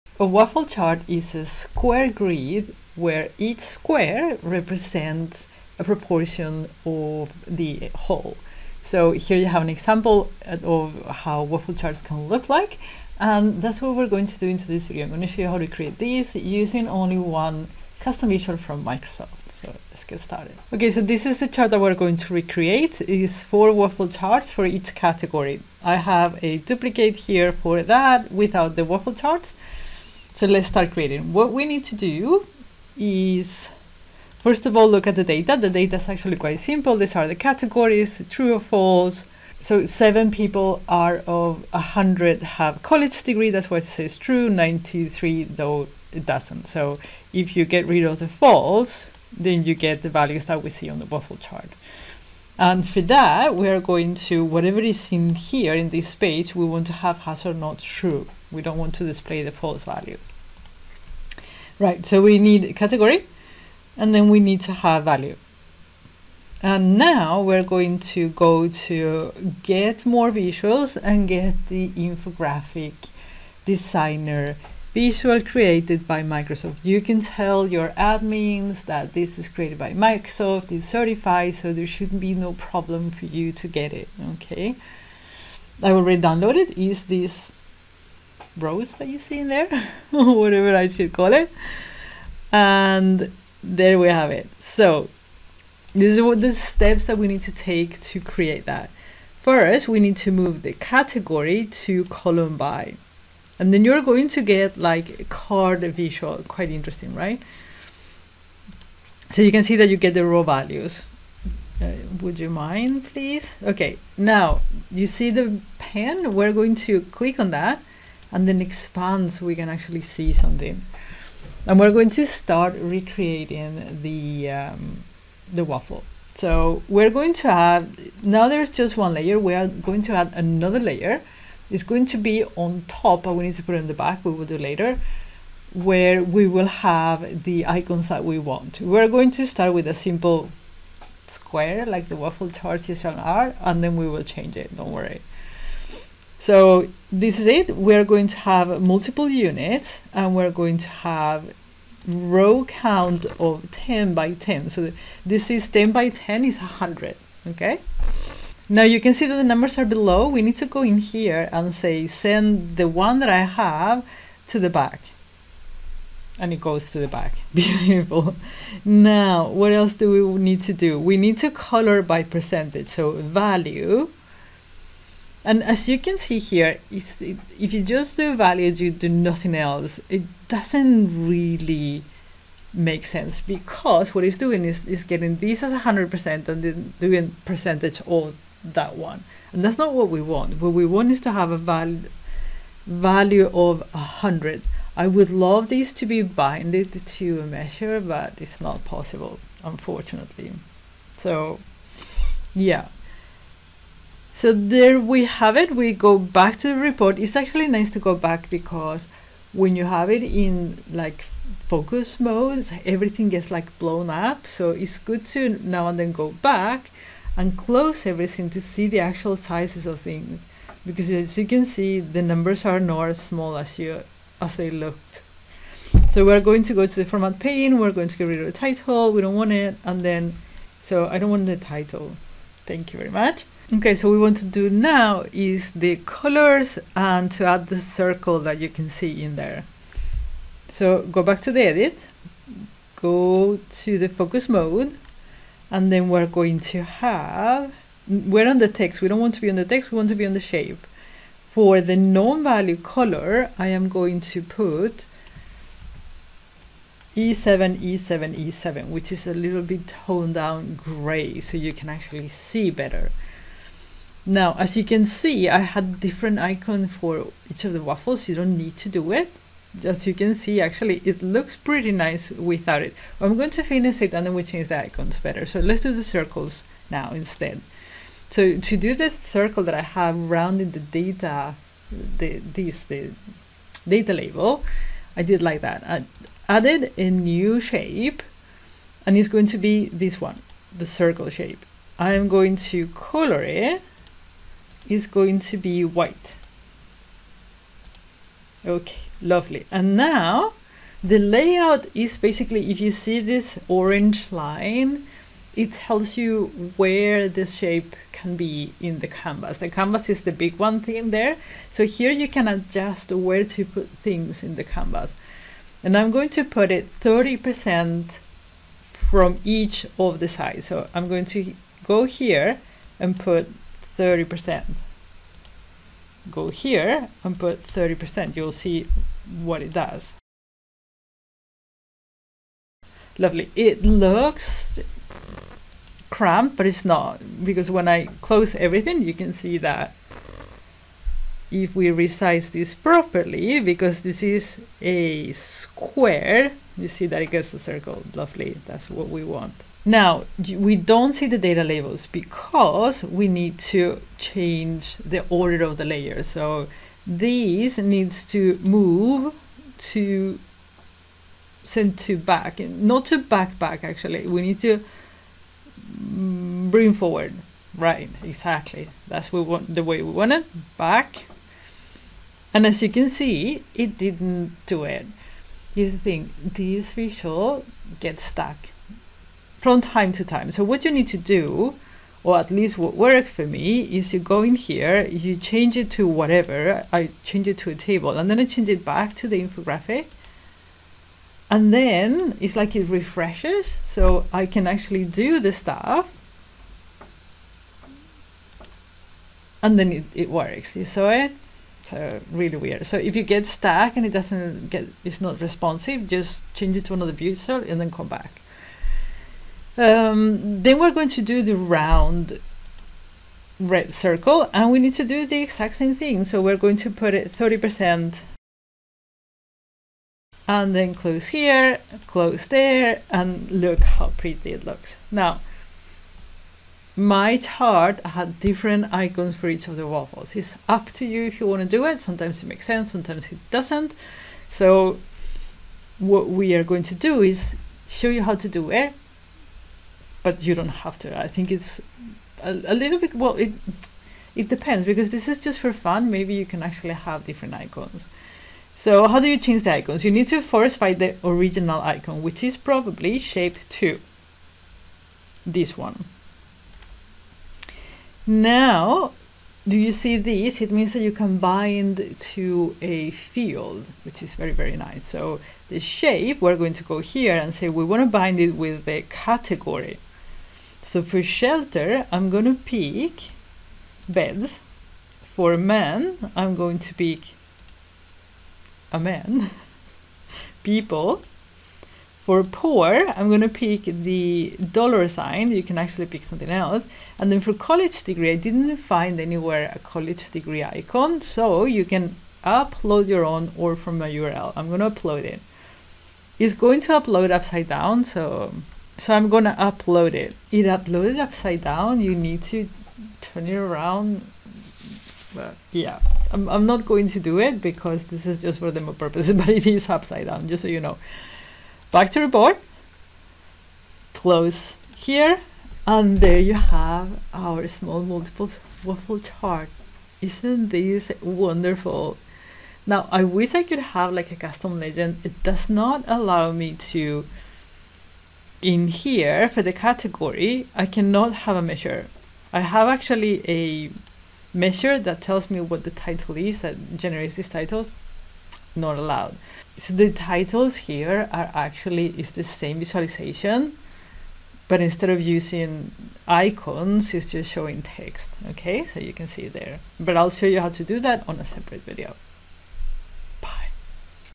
BEAUTIFUL and EASY infographics in Power BI | Waffle charts
Learn how to create visually appealing and informative waffle charts in Power BI with this step-by-step guide.
Waffle charts are perfect for displaying percentage data in a grid format, making it easier to understand proportions at a glance. '
This tutorial covers everything from importing custom visuals to configuring data and formatting the chart for optimal clarity.